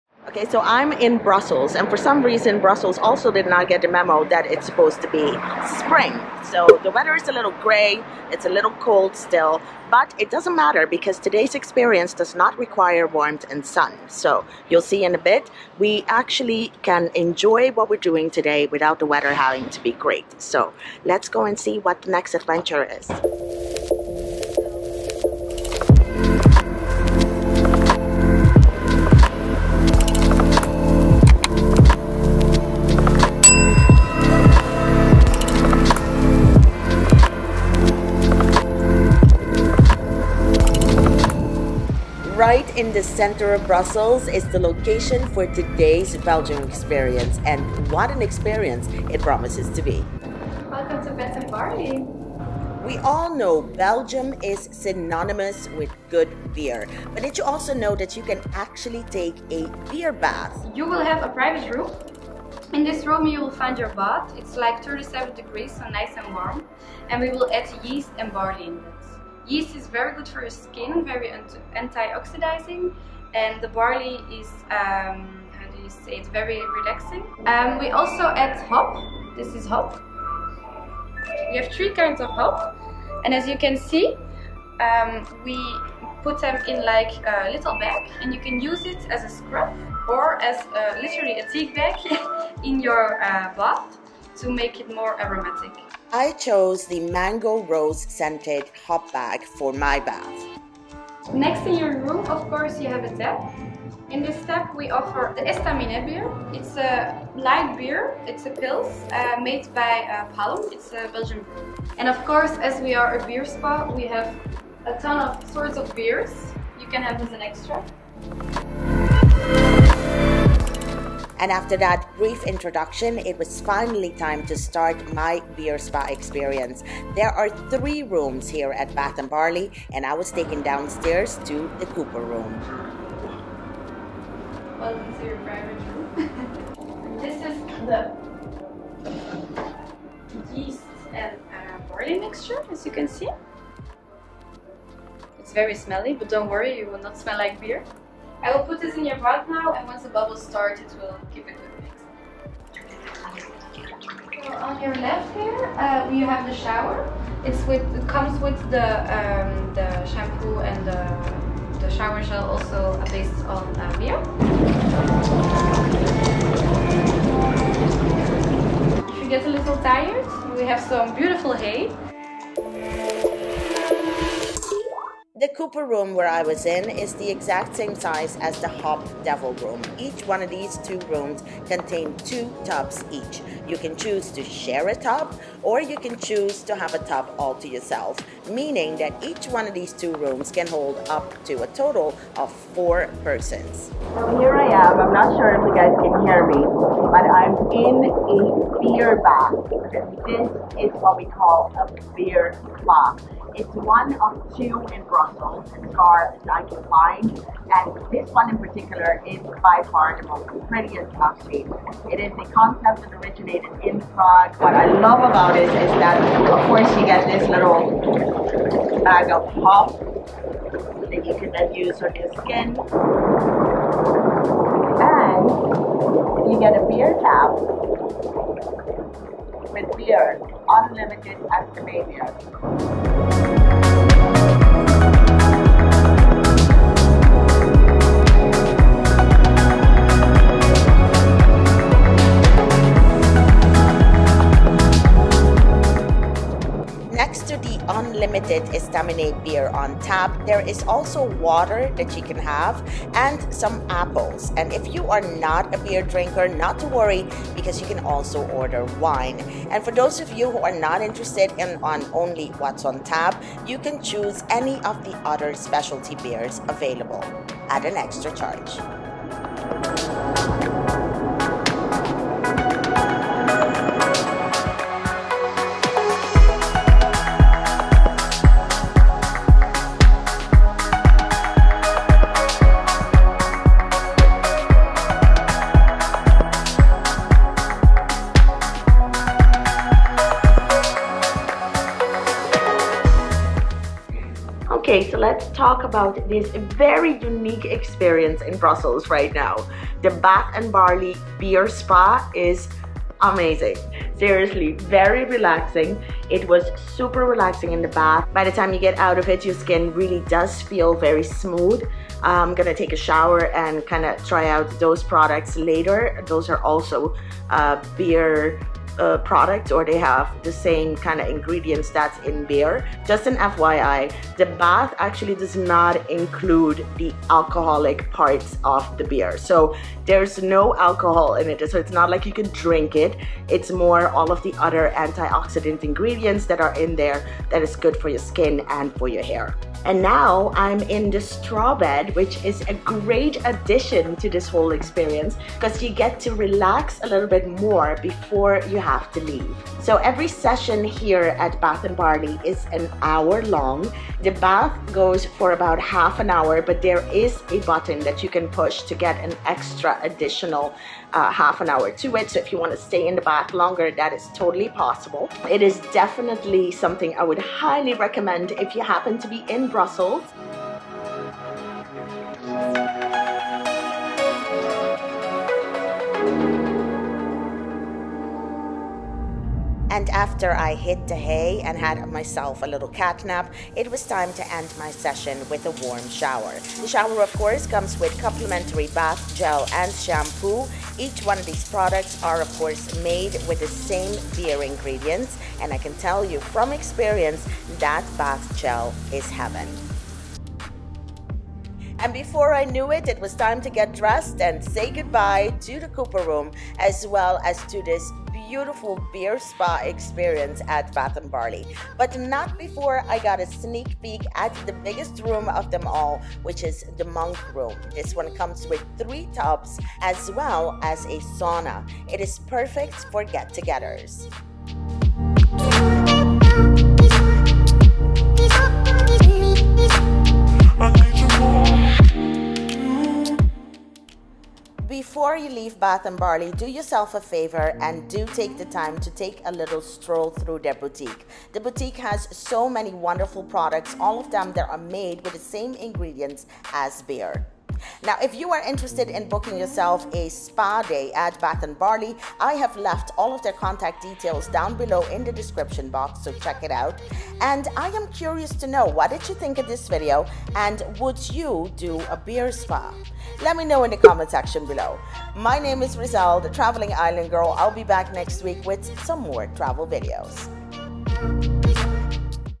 I took a Bath in Beer! - Ultimate Beer Bath Experience at Bath &B Barley
Bath & Barley opened its doors to the public on May 1st, 2021 to offer a very unique experience. The experience of soaking in beer!
Bath & Barley's concept allows you to bathe in a jet tub filled with warm beer, drink unlimited beer on tap from the tap next to the bath (Yes, you don't even need to get out of the tub to fill your glass) and a bed of hay where you can lay down and relax after your soak.
Would you do this Beer Spa when in Brussels?